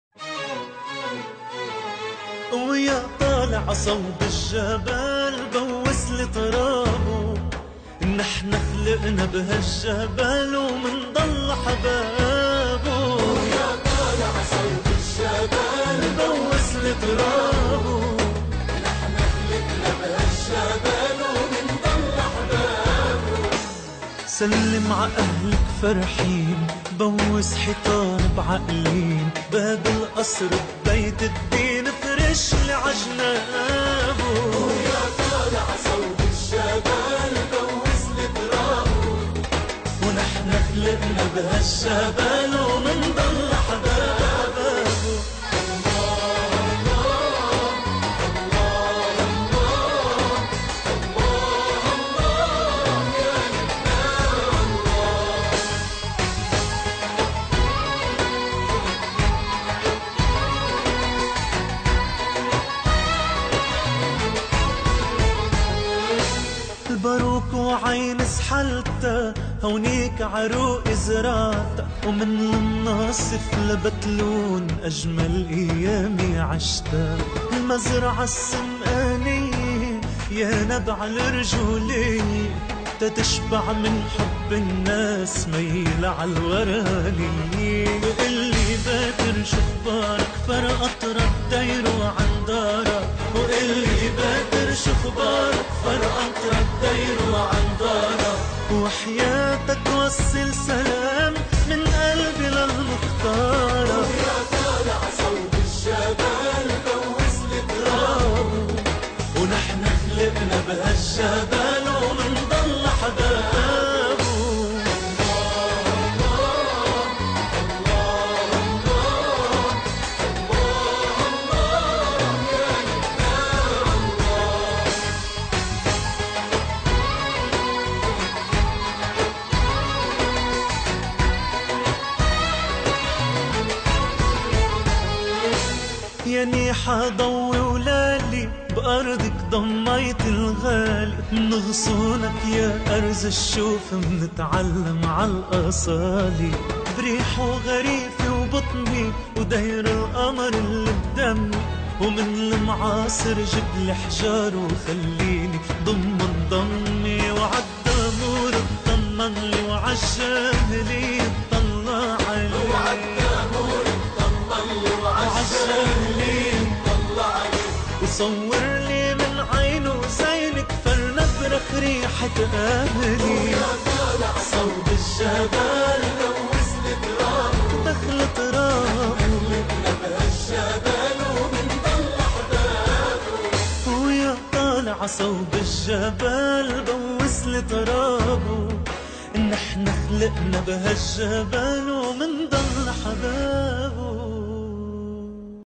Hiking in Dmit, Bikaaya, Fekhayte, Serjbeil, Chamaarin and Jehlieh🇱🇧❤
Thank you Born2hike
Guides: Zahi Ramadan and Joseph Sfeir